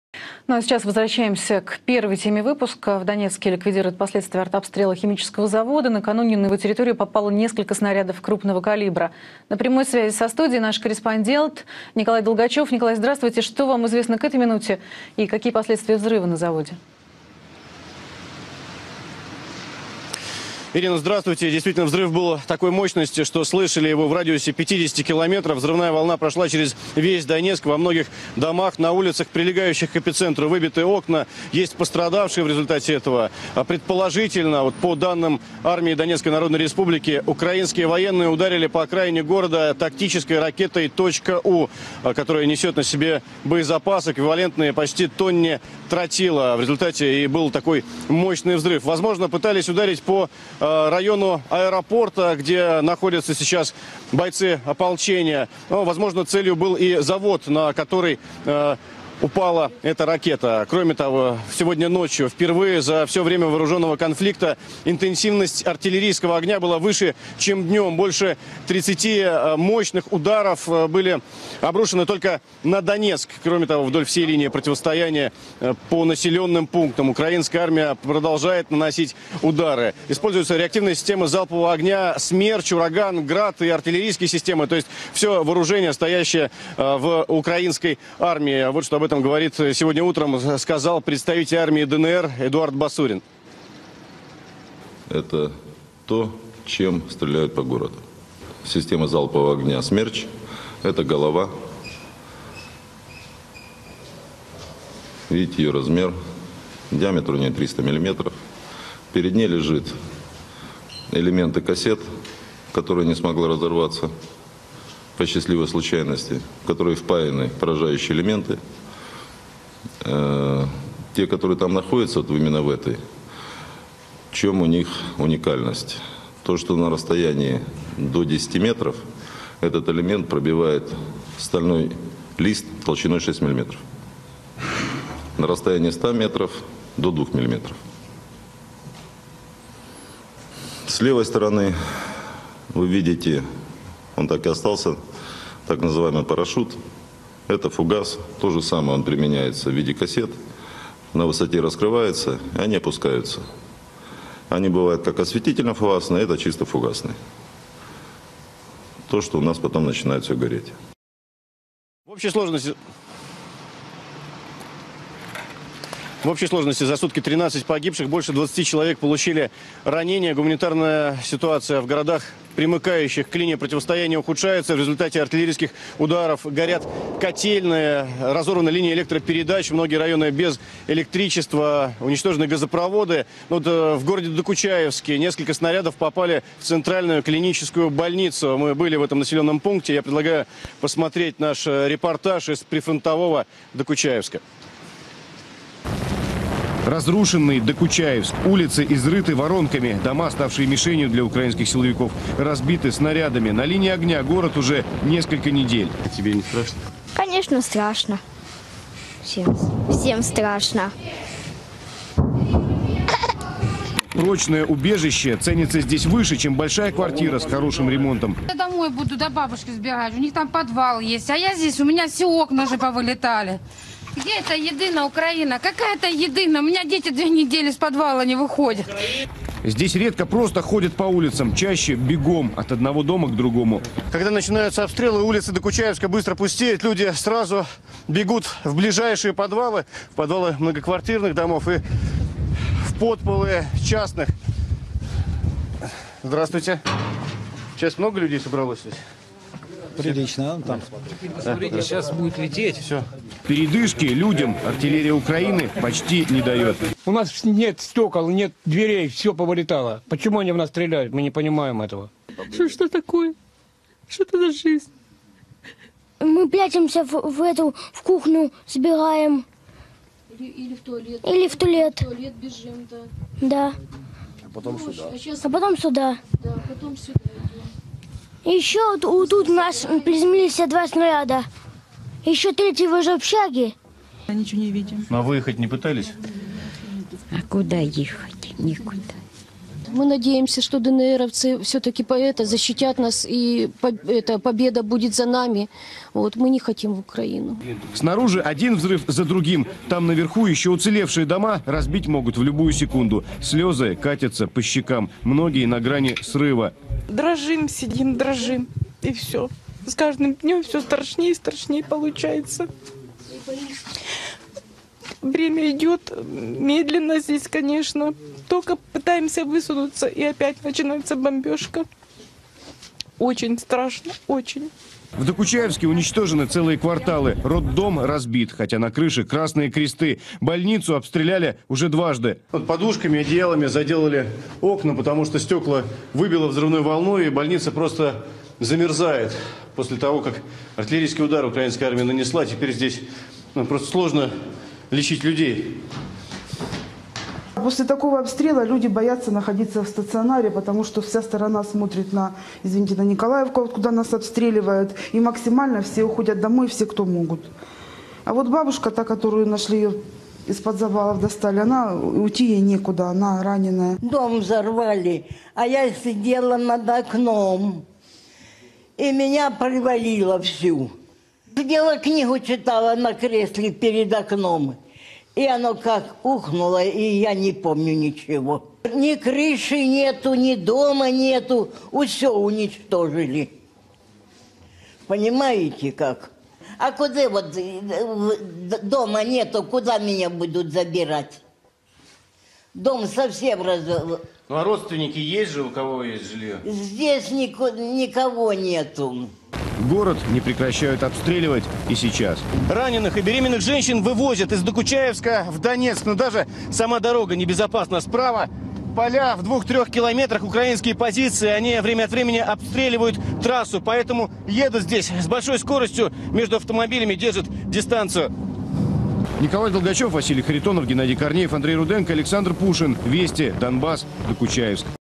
Минобороны ДНР: на взорвавшемся химзаводе в Донецке хранились боеприпасы.
9 февраля 2015.

Представитель минобороны Донецкой народной республики Эдуард Басурин подтвердил взрыв на Донецком казенном заводе химических изделий, произошедший ночью. По его словам, там находились боеприпасы. Данных по поводу нахождения там химических веществ у Басурина нет.